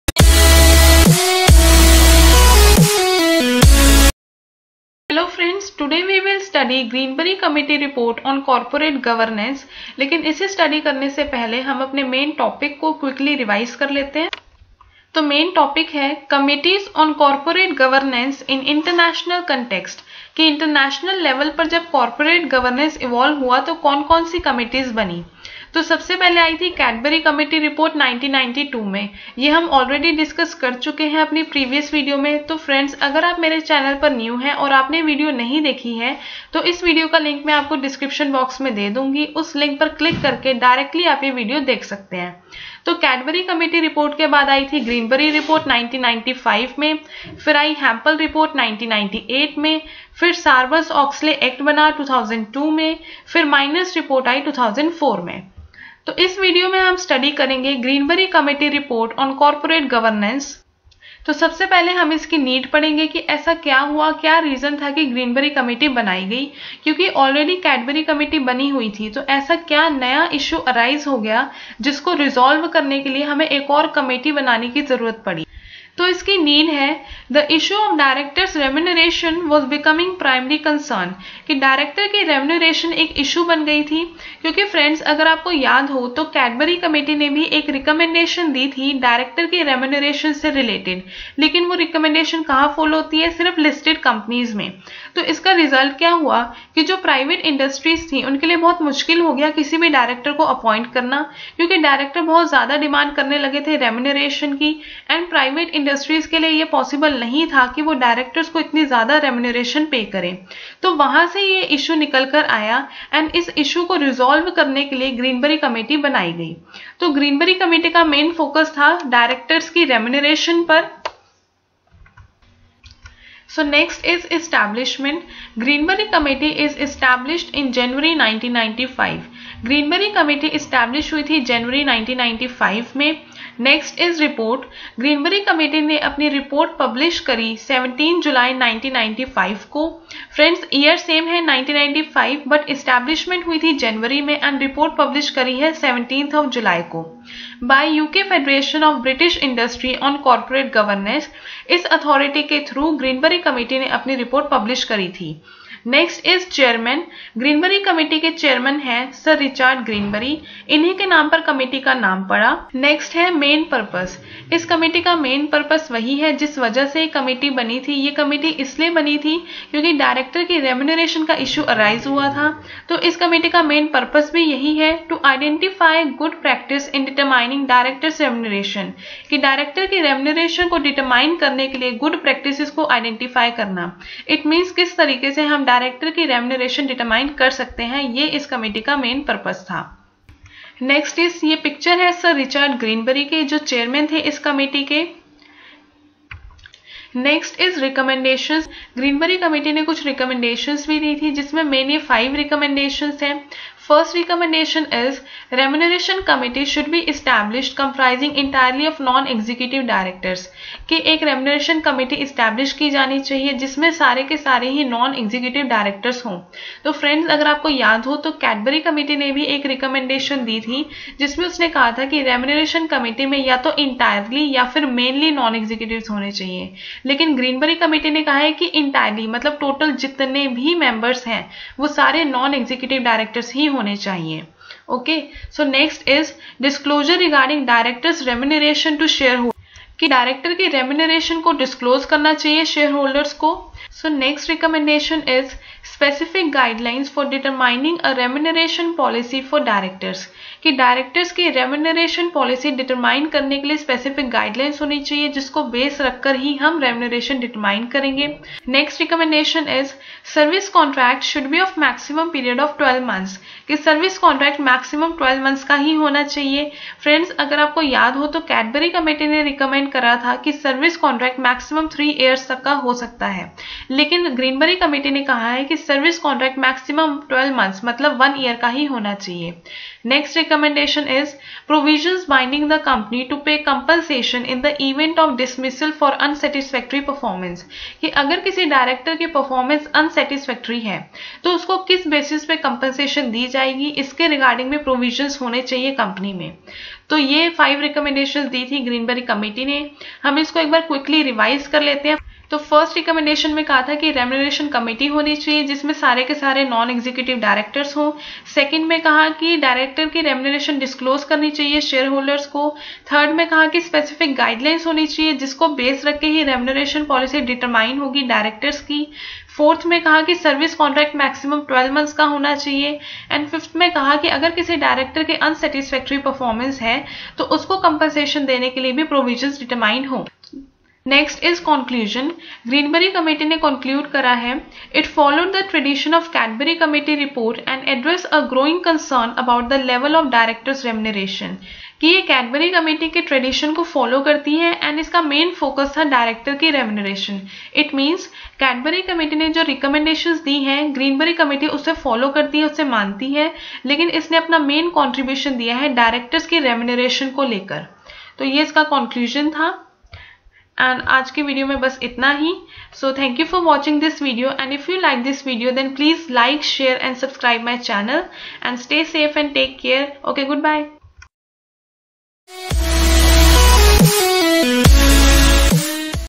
Greenbury Committee Report 1995 on Corporate Governance
Committees and their recommendations on corporate Governance in international context

Introduction:- Greenbury Committee is established in January 1995.
Report:- published in 17 July 1995
Chairman :- Sir Richard Greenbury.
Purpose:- to identify good practices in determining directors remuneration.
This report has given main 5 recommendations that we had discussed in this video.
corporate governance governance.